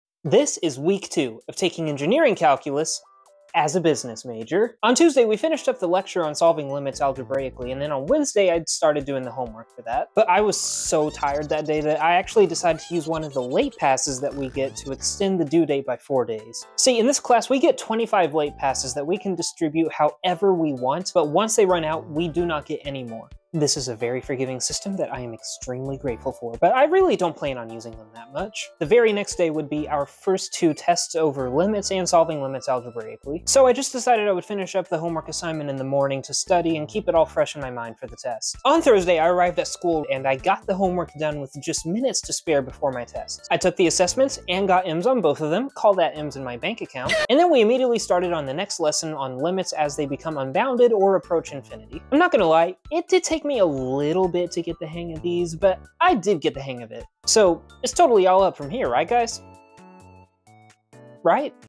Week 2 of Engineering Calculus as a Business Major
It's finally winter break so I'm going to be editing all the footage I got for the semester and making these videos as fast as I can.